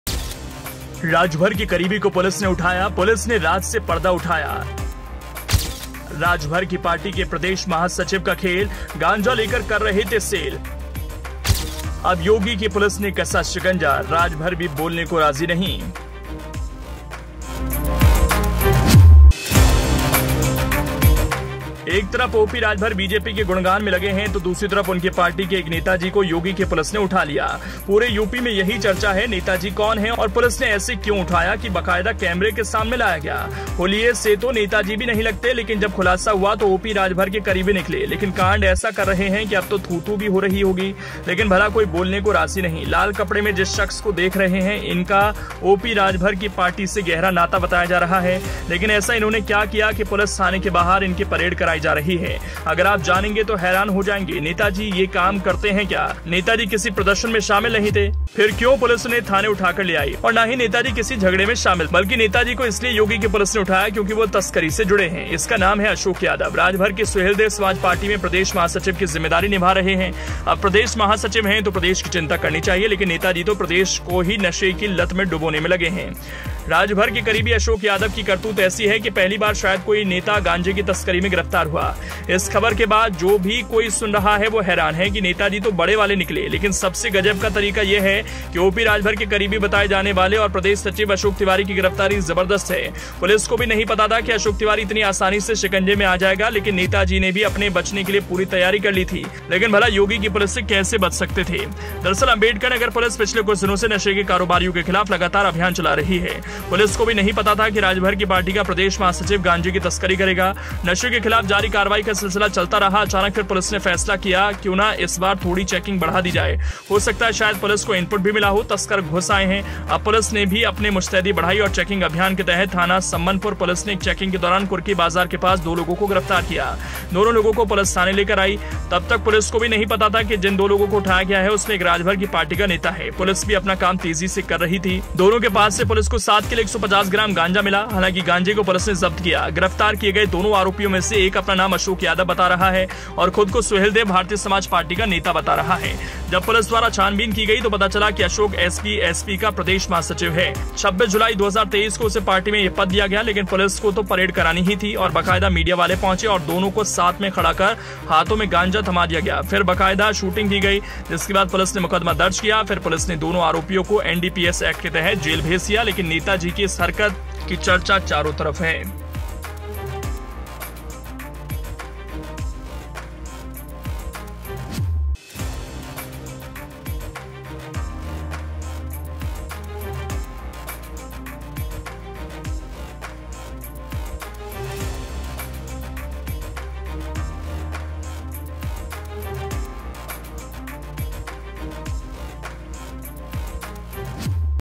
Yogi की पुलिस का OP Rajbhar के 'खास' पर चला चाबुक | The Rajneeti
Yogi की पुलिस का Om Prakash Rajbhar के 'खास' पर चला चाबुक... Akhilesh Yadav ने कसा तंज- गांजे को शौकीन  हैं  !